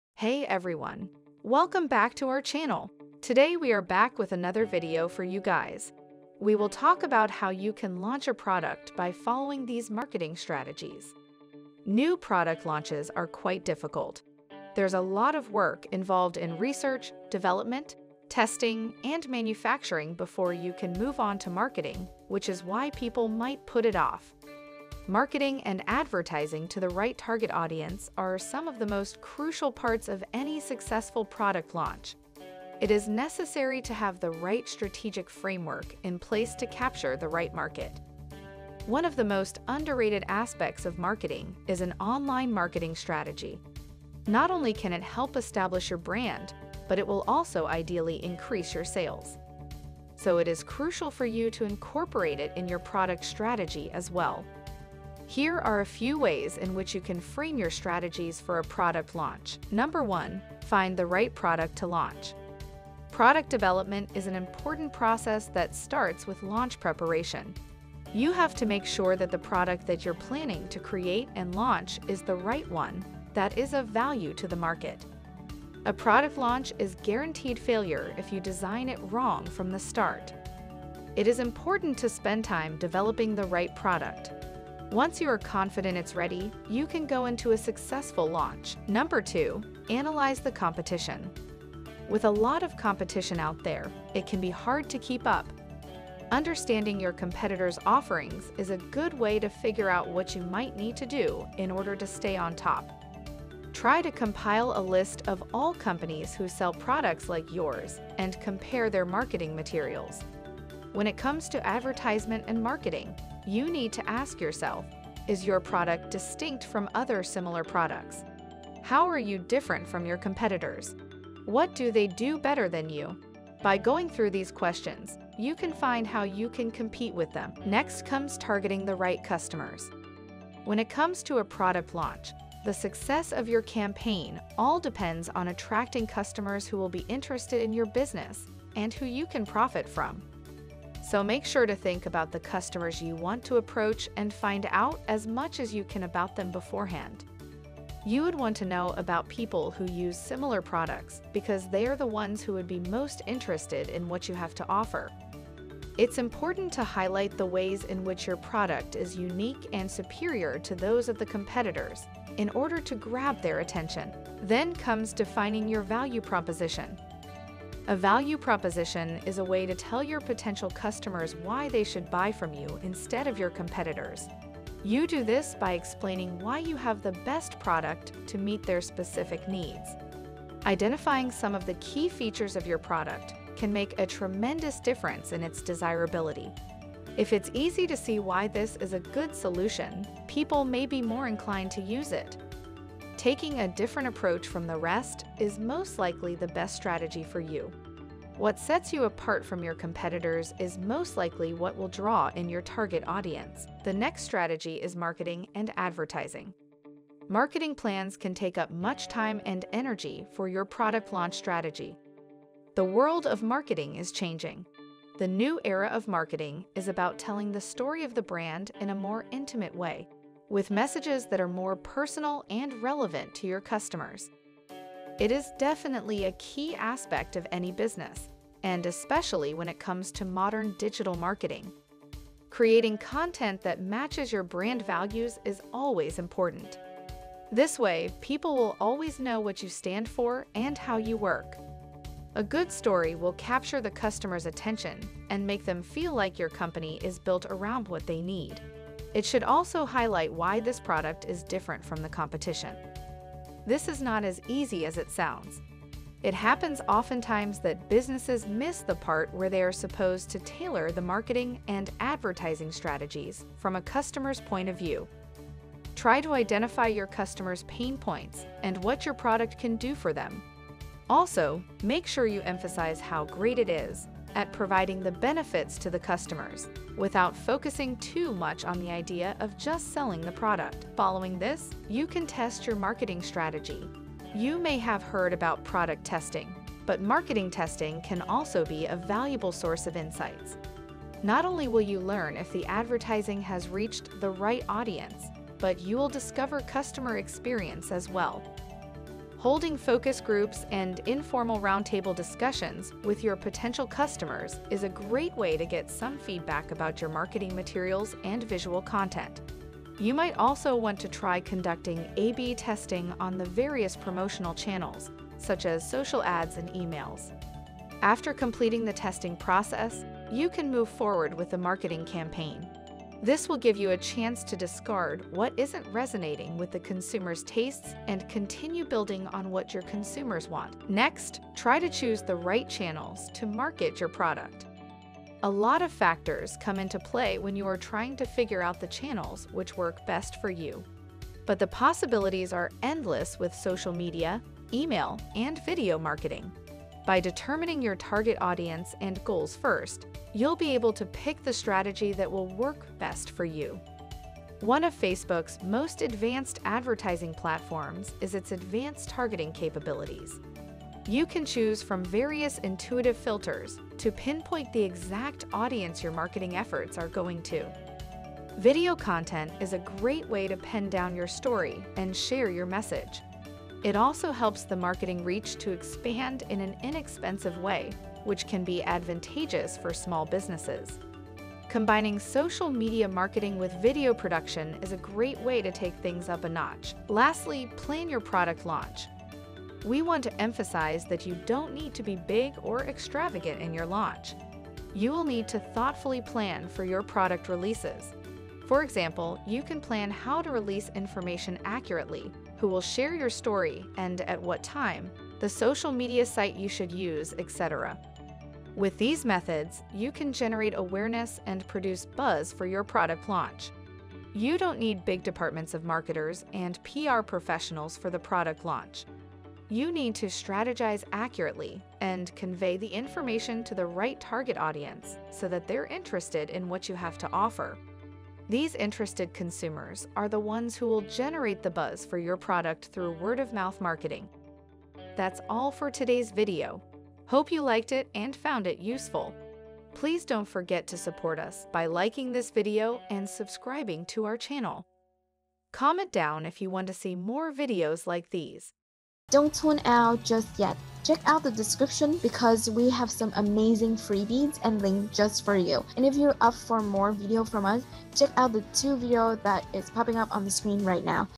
Proven Product Marketing Formula For 2023 (+ Secret Product Launch Hacks)
Planning a product launch strategy is an integral part of any successful venture.

Whether you're just starting out or want to rebuild the foundation of your company, you'll need to come up with an effective product marketing strategy that includes pivotal questions and many factors for consideration.

In this video, we will tell you a few important steps that you can use as a part of your product marketing strategy. Follow these steps in your marketing plan to achieve the desired goals and reach the next level!

Check out our MAD GROWTH ACCELERATOR (Free Course on digital Marketing)

JOIN STANDOUT PRO Membership and get access to resources worth $10k USD